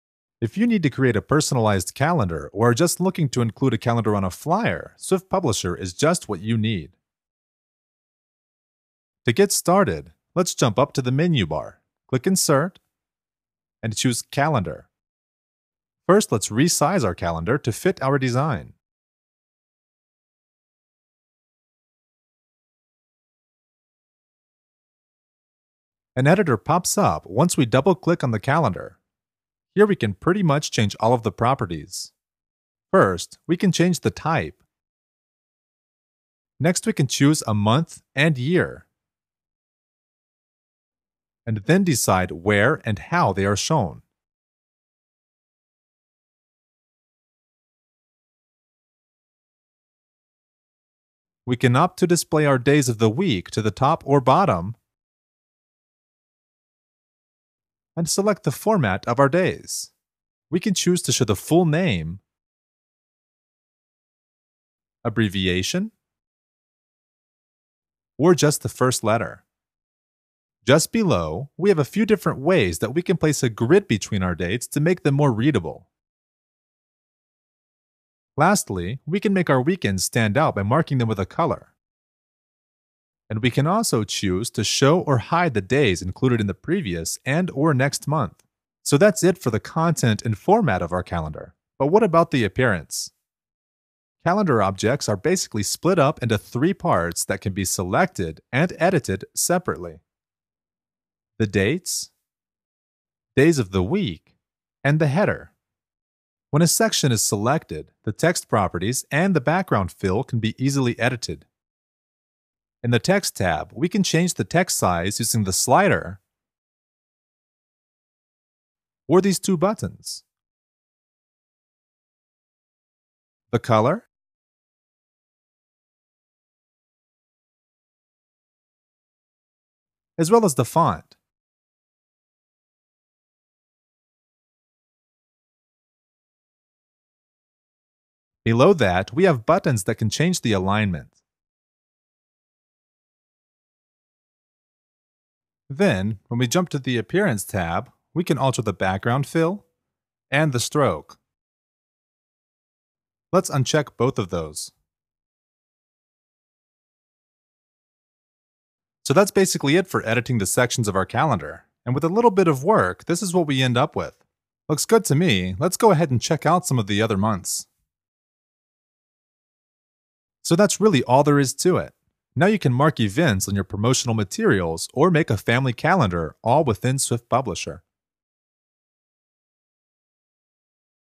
Swift Publisher 3 Tutorials - Adding and Editing Calendars
Swift Publisher is a sleek desktop publishing app built for the Mac. Now creating flyers, brochures, calendars, newsletters, and other similar documents is a breeze.

This tutorial shows you the basics of creating and editing calendar objects in Swift Publisher 3.